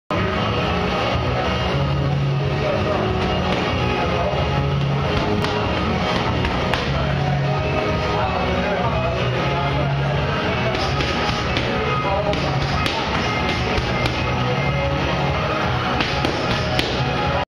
Slider of Juan
Juan Rogel of Puro Curazón Boxing program helps out with our sizzle clip.